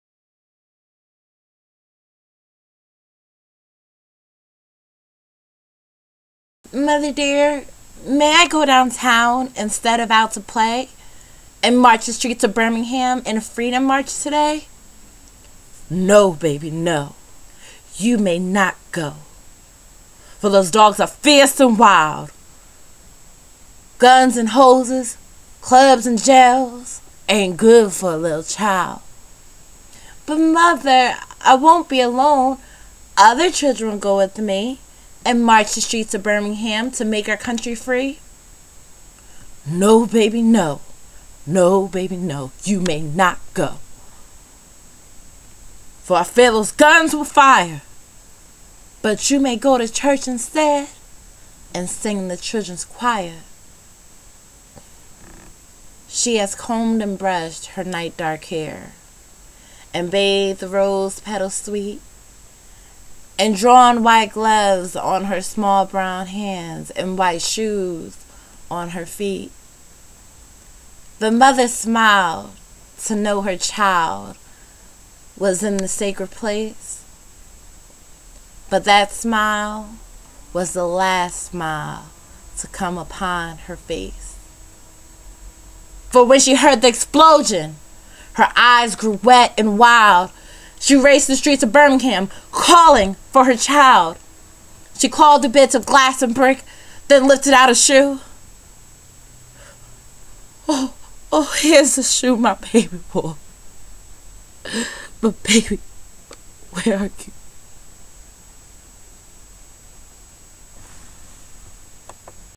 The Ballad of Birmingham by Dudley Randalld
The Ballad of Birmingham recited by Aisha Davis